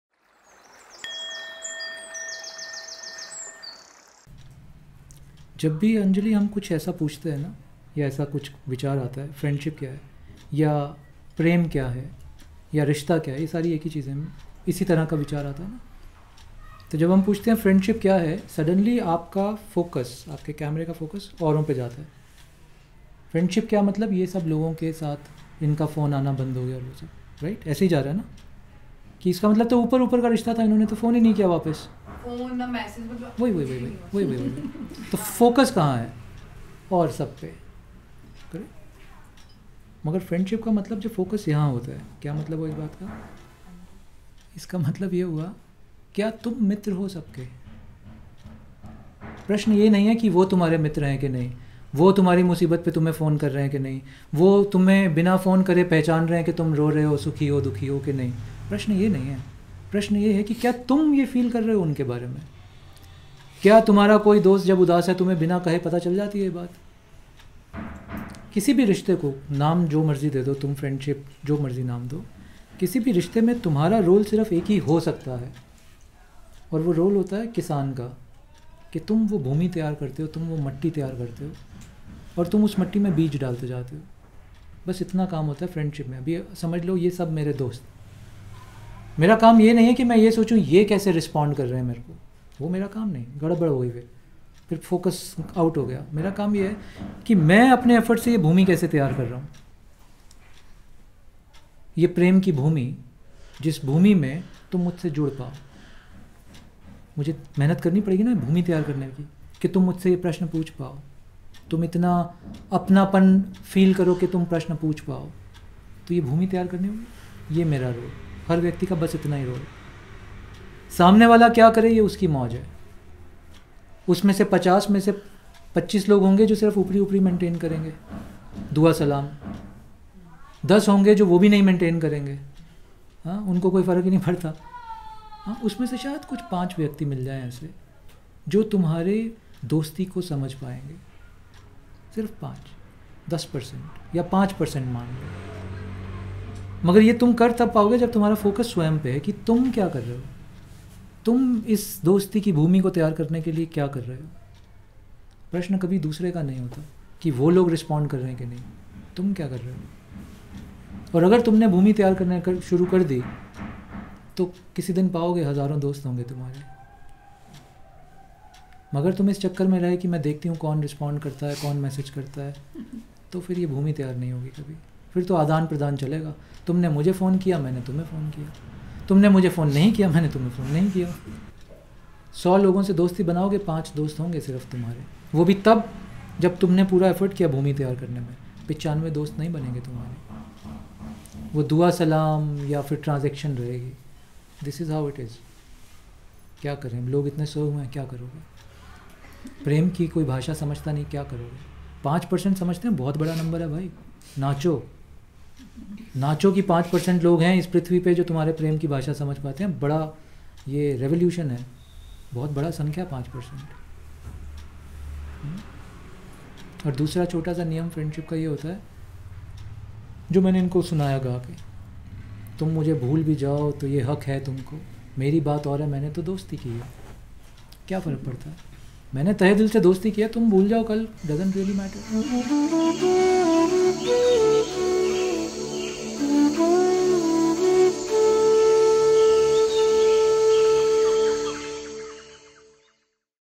असली मित्रता की पहचान क्या है ? Mustard Seeds | Sri Anish | Saadho
असली मित्रता की पहचान क्या है ? यह प्रशन अक्सर हमें परेशान करता है। इस वीडियो मैसेज में श्री अनीश 4 मिंटो में इस विषय पर समझाते हैं |